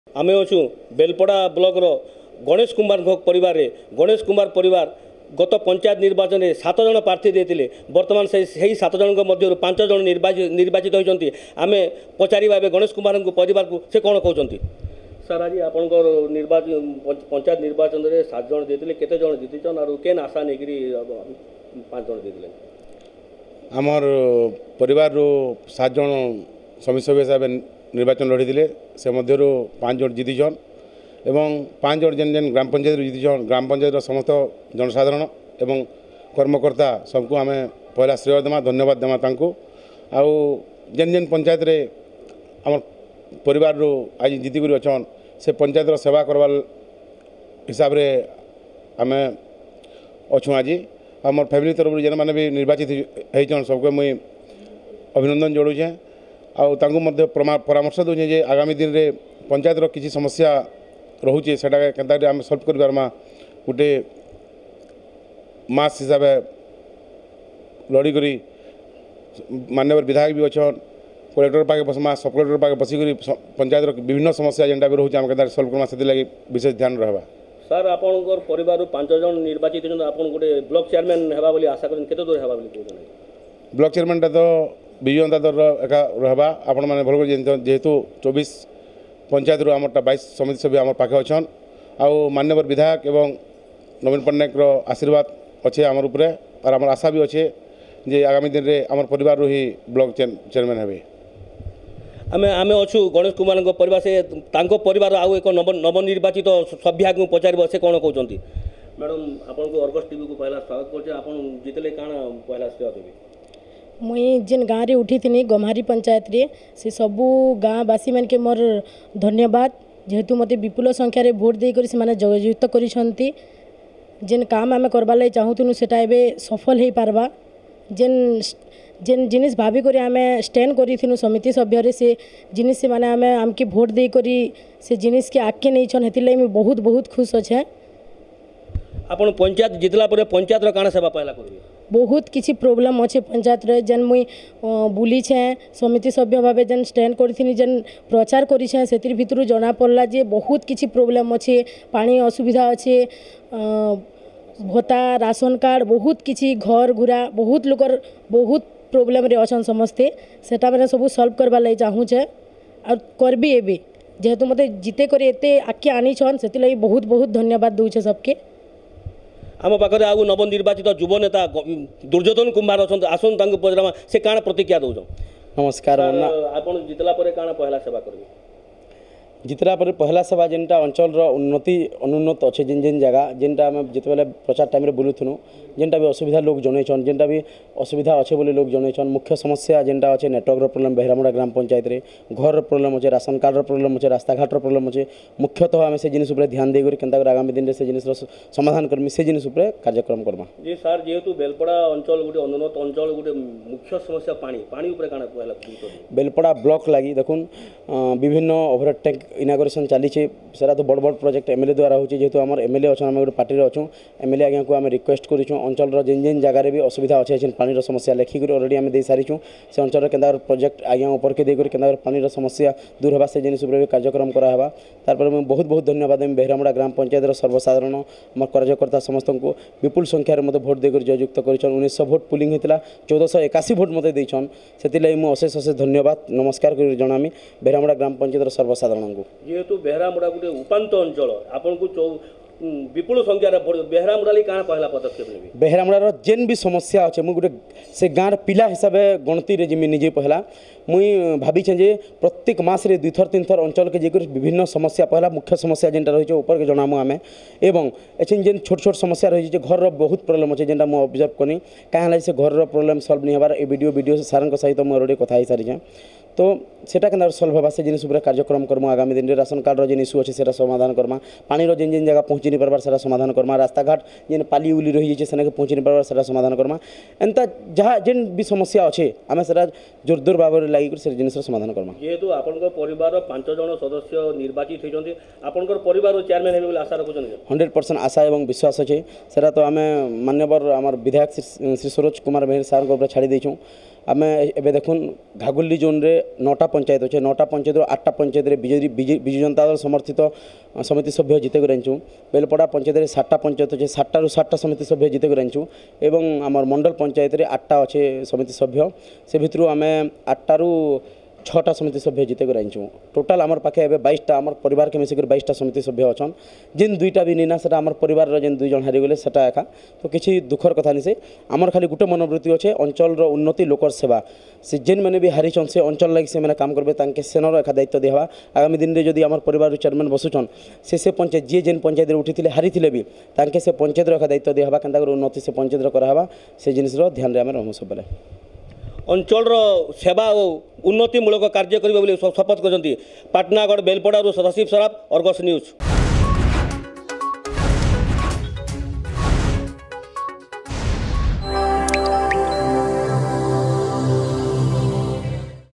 Election Result 2022: 7 Members of a Family fight for Panchayat Samiti Seat, 5 Win in Belpada Block
In the panchayat elections, seven members of Ganesh Kumhar's family were fighting as candidates for the Panchayat samiti Seat of the Belpada block in various panchayats. Five of them have won the election.